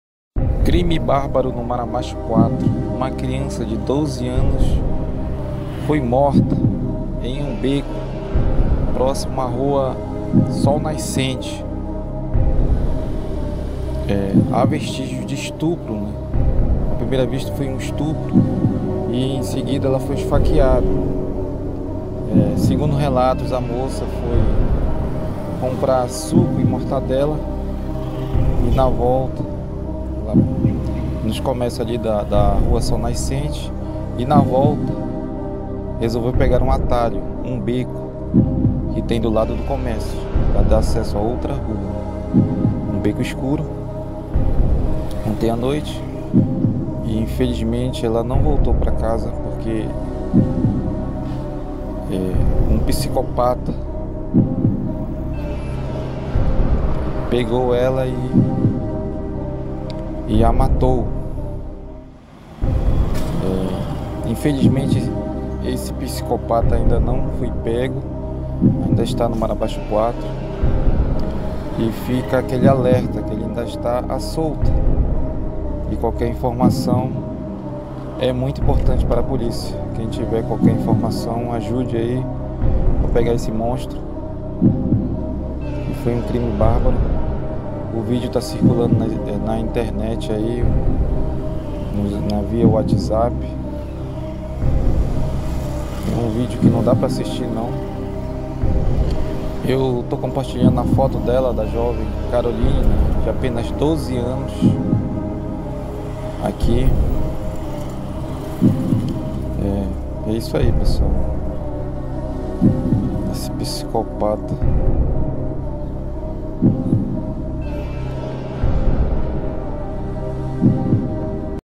Crime bárbaro no Marabaixo4 ,zona oeste de Macapá_Caroline Miranda de apenas 12 anos.
Assassinada na noite de 30/11/2021.
Qualquer informação do assassino ligue 190.